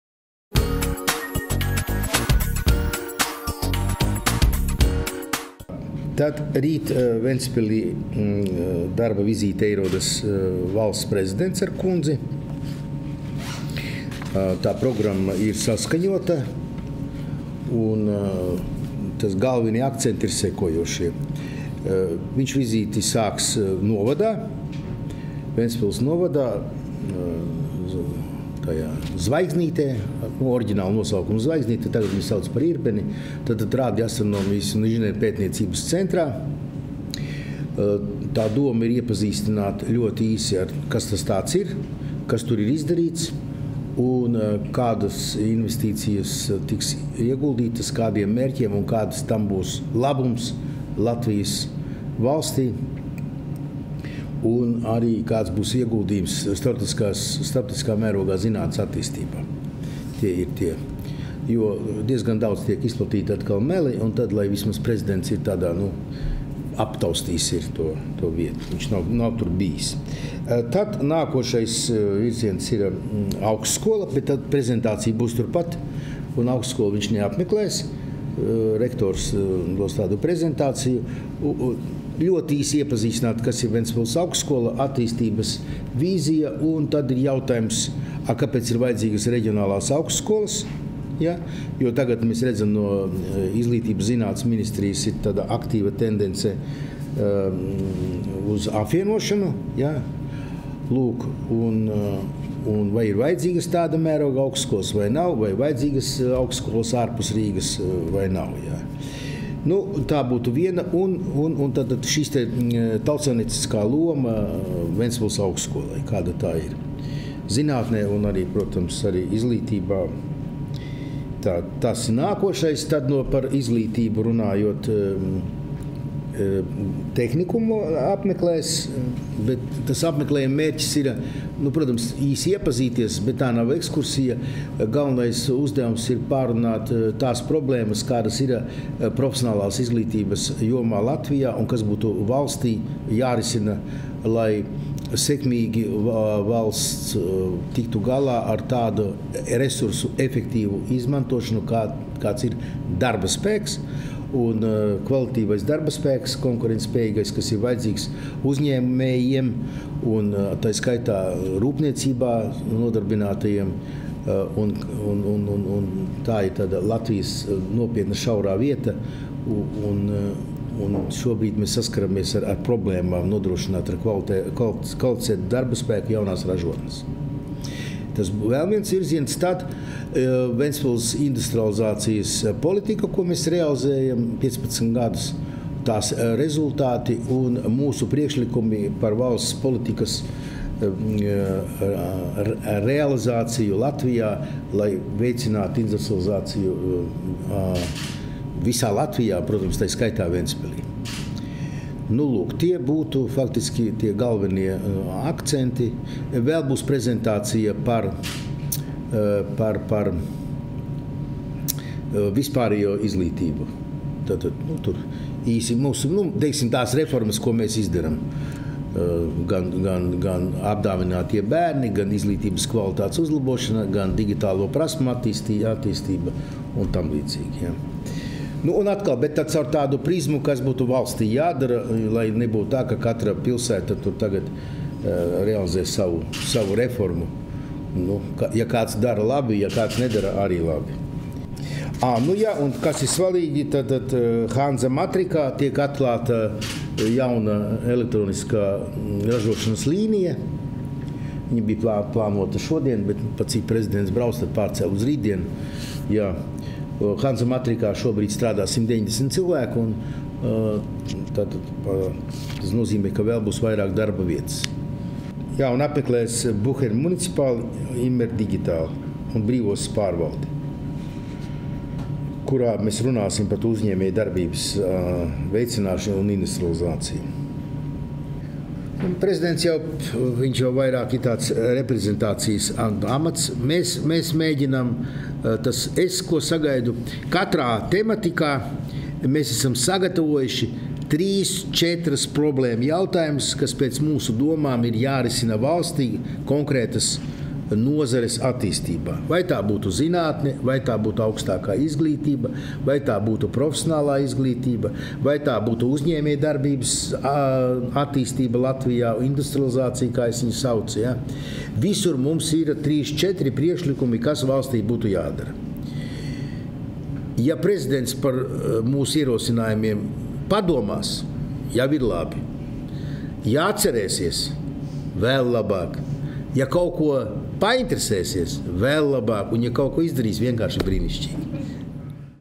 Lembergs: Rīt darba vizītē Ventspilī ieradīsies LR valsts prezidents ar kundzi!
Savā iknedēļas preses konferencē Ventspils pilsētas domes priekšsēdētājs Aivars Lembergs stāstīja par ceturtdien, 23.februārī, plānoto Latvijas Republikas Valsts prezidenta Raimonda Vējoņa un Ivetas Vējones kundzes darba vizīti Ventspilī.